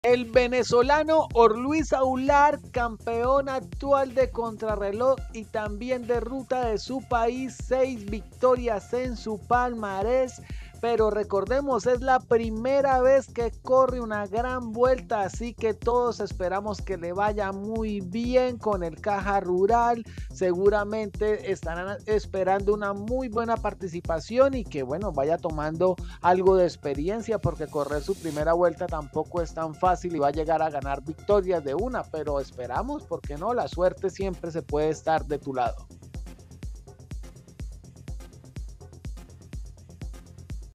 "ORLUIS AULAR: Determinación y Fuerza en la Vuelta a España 2023" CICLISMO VENEZOLANO
Explora la determinación y la fuerza de Orluis Aular en la Vuelta a España 2023, representando a Venezuela con pasión. Analizaremos su desempeño en las etapas y cómo se enfrenta a los retos del pelotón internacional. Únete para conocer cómo este talentoso ciclista venezolano se destaca en la competición de élite. 🚴‍♂️💪🏆